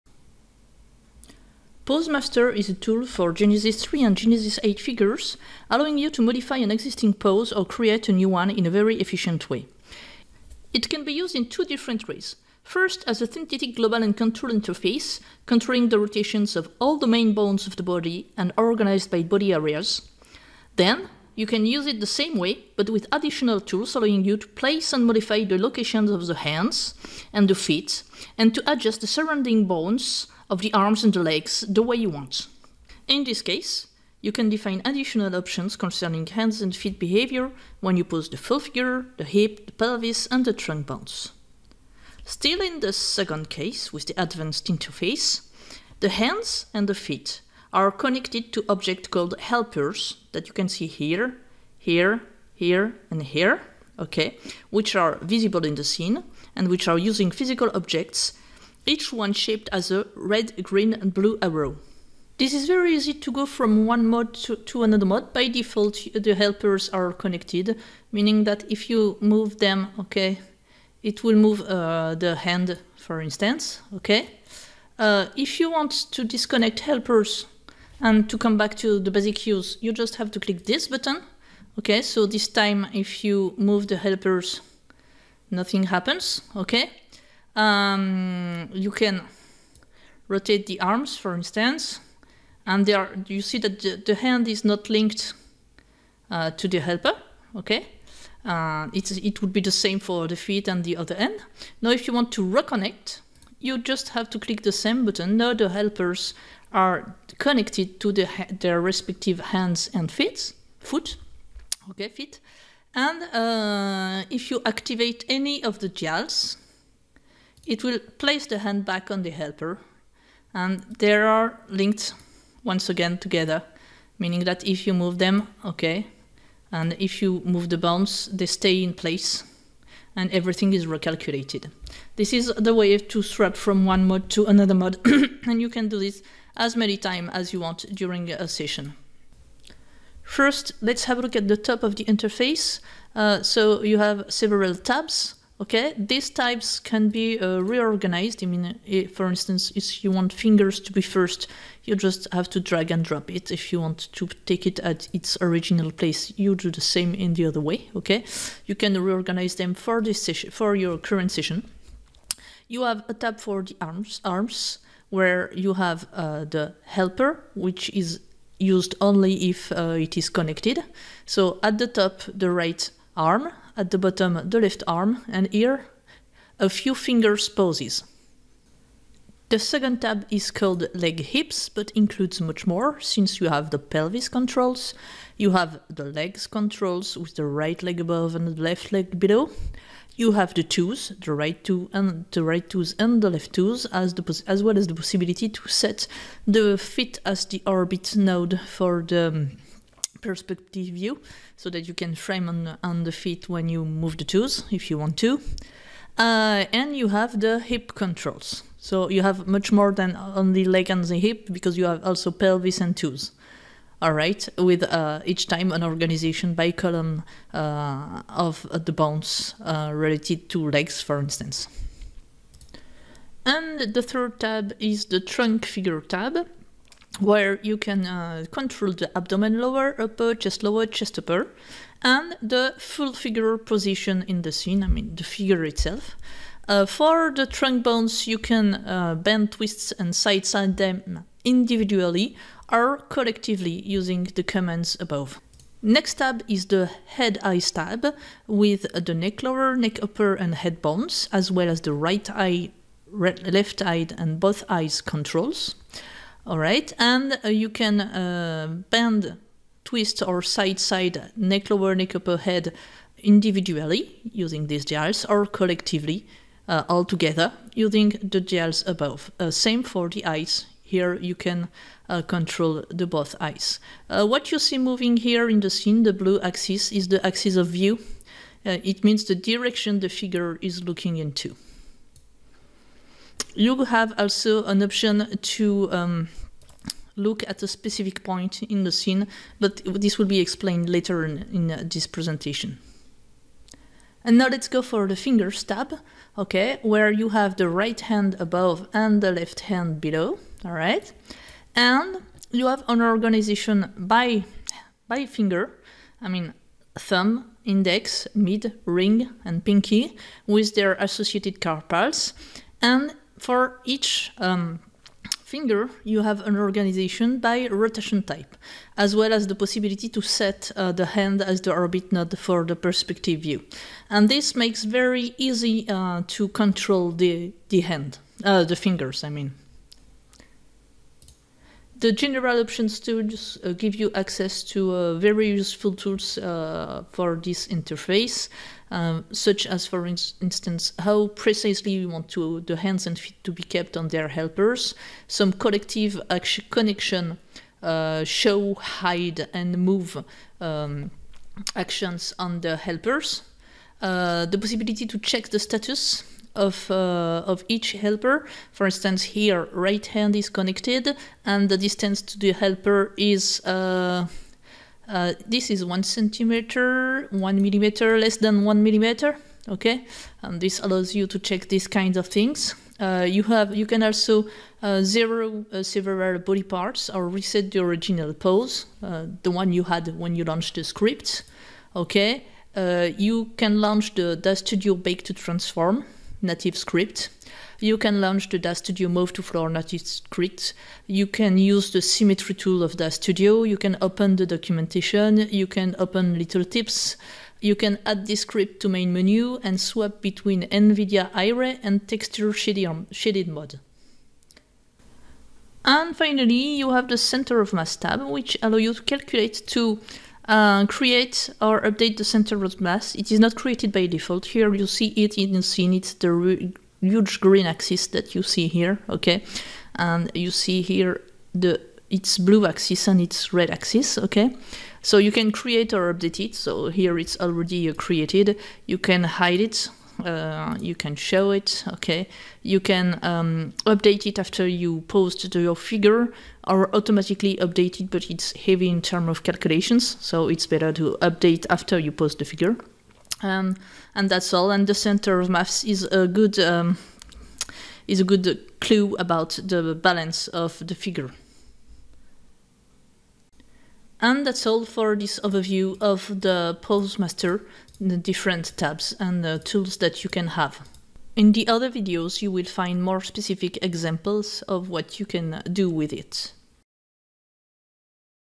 Pose Master 1 INTRODUCTION AND TABS OVERVIEW
This is a rapid overview of Pose Master Tabs and Interface. More videos are available to have more details on Pose Master